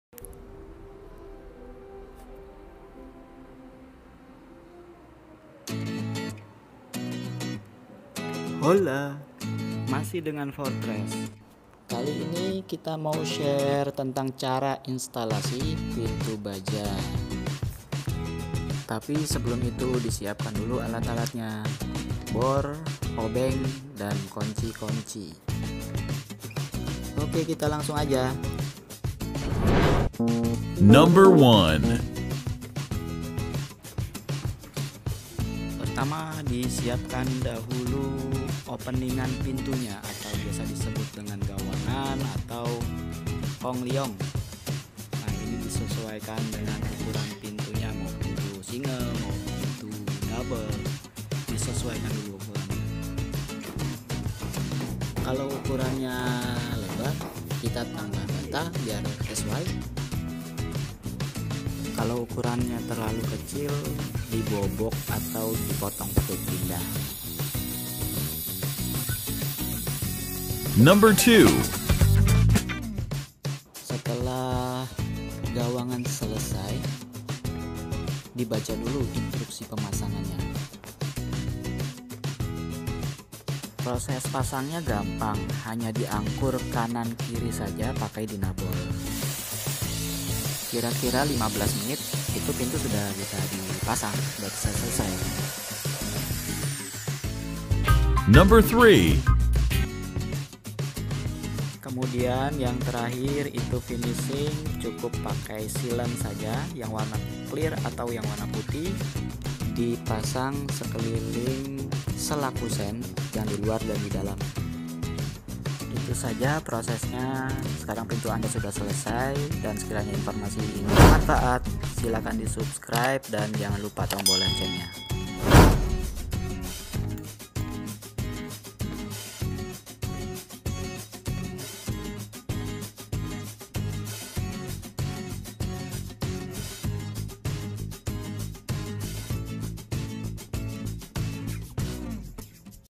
Cara Pasang Pintu Baja | 0812-9219-2703
Persiapan & Alat yang Digunakan;
1. Mesin Bor, Obeng & Kunci
2. Sealent & Tembakan nya
3. Palu & Pahat Beton
4. Sendok Semen

3 Cara Mudah Pemasangan Pintu Baja
- Persiapan Openingan/Gawangan
- Pemasangan Pintu
- Finishing

Segitu dulu ya kak,
Detailnya pilih aja mau pakai channel mana, senyaman nya aja.